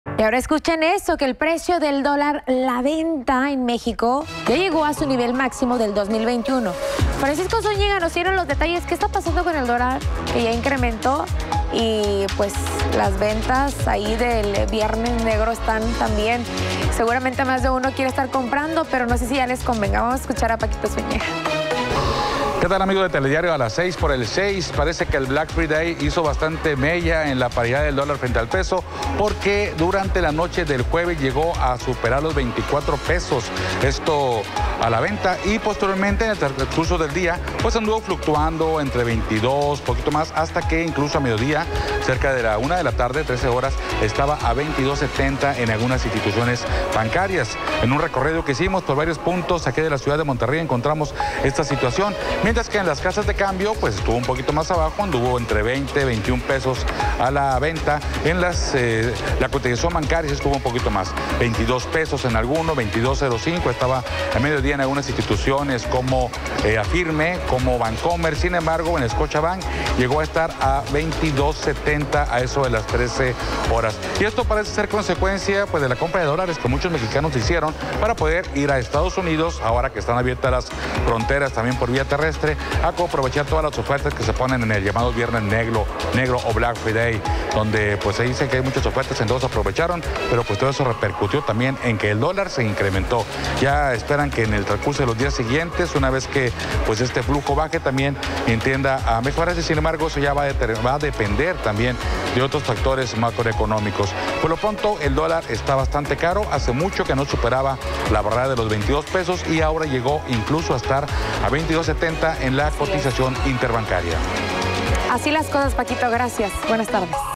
Dólar se cotiza en bancos hasta en 22 pesos
Al iniciar operaciones, el precio del dólar hoy viernes fue de hasta 22.08 pesos en bancos. En tanto, el tipo de cambio interbancario se ubica en 21.82 pesos por dólar, con una pérdida de 1.23 por ciento, frente a los 21.55 del precio de referencia del jueves.

Programación
Lunes a viernes:
Telediario Matutino de 05:00- 10:00
Telediario Mediodía de 12:00-14:30
Telediario Nocturno de 19:00- 21:00